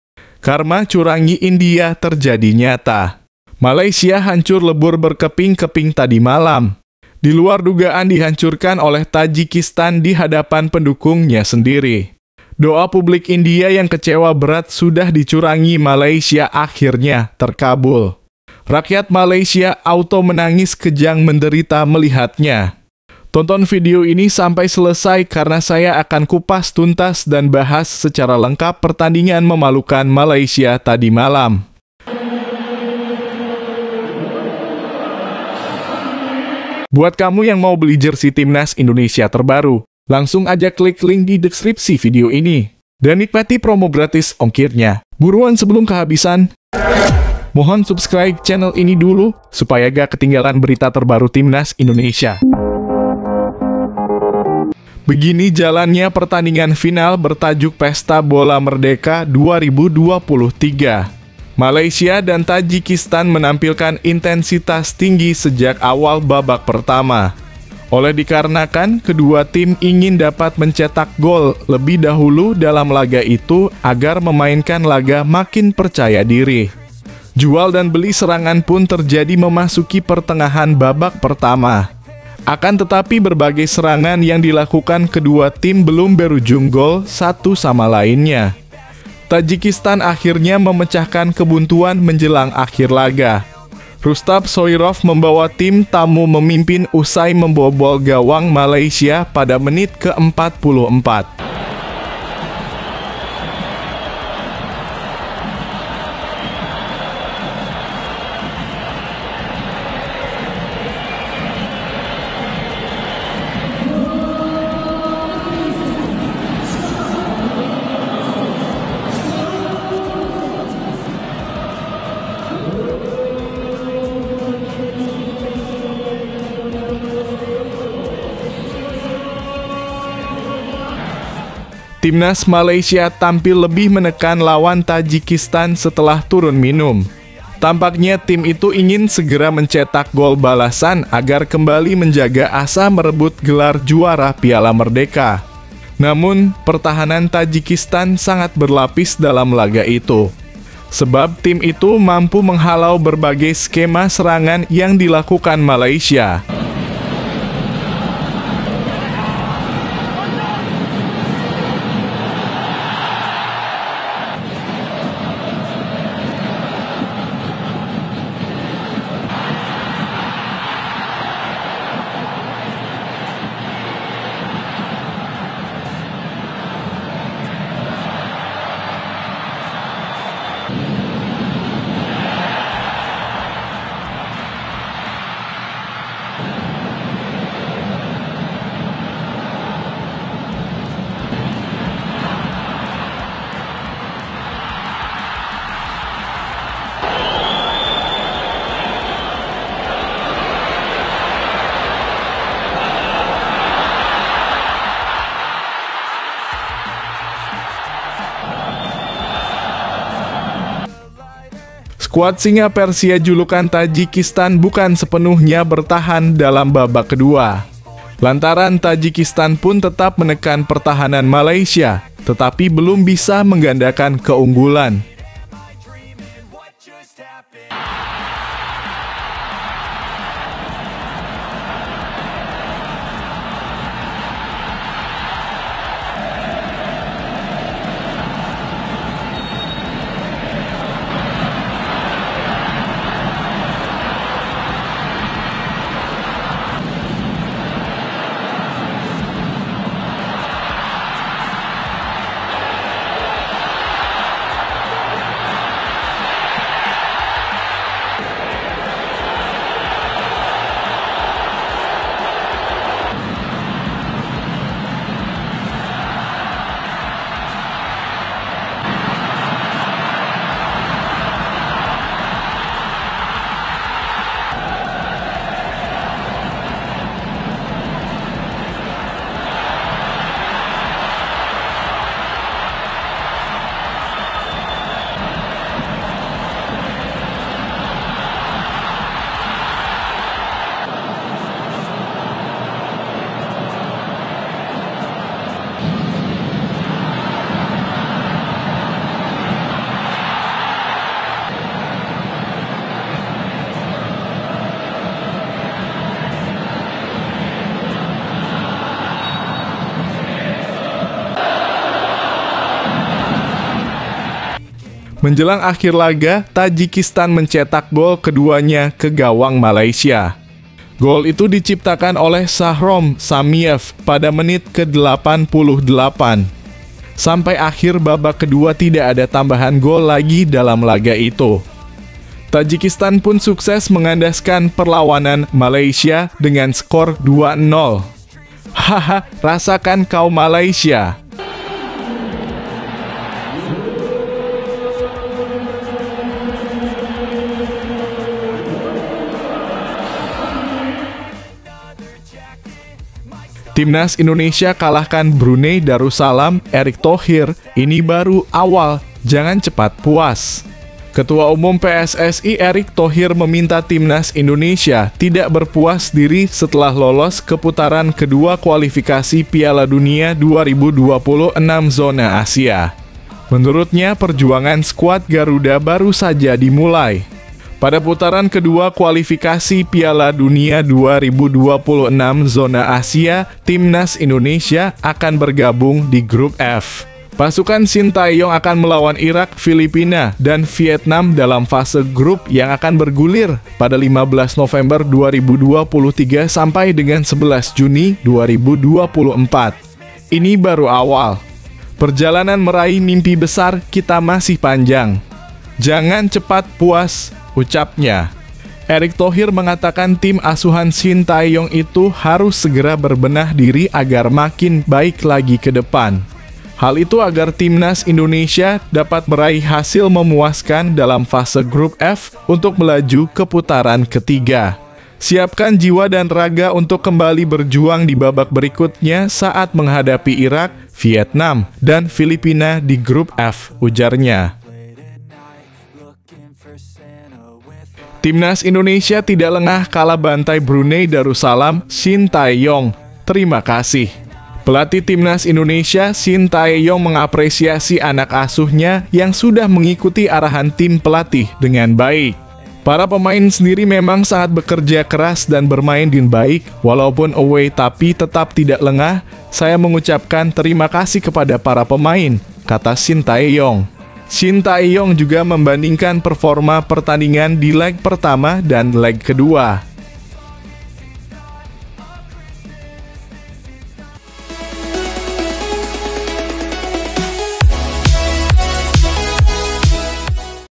🔵 DOA PUBLIK INDIA TERKABUL ‼️ Timnas Malaysia KALAH MEMALUKAN Tadi Malam Lawan Tim LEMAH Tajikistan
Karma Curangi India Terjadi Nyata. Malaysia Hancur Lebur Berkeping Keping Tadi Malam. Diluar Dugaan Dihancurkan Oleh Tajikistan di Hadapan Pendukungnya Sendiri. Doa Publik India Yang Kecewa Berat Sudah Dicurangi Malaysia Akhirnya Terkabul. Rakyat Malaysia Auto Menangis Kejang Menderita Melihatnya. Tonton video ini sampai selesai karena saya akan kupas tuntas dan bahas secara lengkap pertandingan memalukan malaysia tadi malam.

Begini jalannya pertandingan final bertajuk pesta bola merdeka 2023. Malaysia dan Tajikistan menampilkan intensitas tinggi sejak awal babak pertama. Oleh dikarenakan, kedua tim ingin dapat mencetak gol lebih dahulu dalam laga itu agar memainkan laga makin percaya diri. Jual dan beli serangan pun terjadi memasuki pertengahan babak pertama. Akan tetapi, berbagai serangan yang dilakukan kedua tim belum berujung gol satu sama lainnya. Tajikistan akhirnya memecahkan kebuntuan menjelang akhir laga. Rustab Soirov membawa tim tamu memimpin usai membobol gawang Malaysia pada menit ke-44. Timnas Malaysia tampil lebih menekan lawan Tajikistan setelah turun minum. Tampaknya tim itu ingin segera mencetak gol balasan agar kembali menjaga asa merebut gelar juara Piala Merdeka.

Namun, pertahanan Tajikistan sangat berlapis dalam laga itu. Sebab, tim itu mampu menghalau berbagai skema serangan yang dilakukan Malaysia. Skuad Singa Persia -julukan Tajikistan- bukan sepenuhnya bertahan dalam babak kedua. Lantaran, Tajikistan pun tetap menekan pertahanan Malaysia, tetapi belum bisa menggandakan keunggulan. Menjelang akhir laga, Tajikistan mencetak gol keduanya ke gawang Malaysia. Gol itu diciptakan oleh Shahrom Samiev pada menit ke-88. Sampai akhir babak kedua tidak ada tambahan gol lagi dalam laga itu. Tajikistan pun sukses mengandaskan perlawanan Malaysia dengan skor 2 0. hahaha rasakan kau malaysia.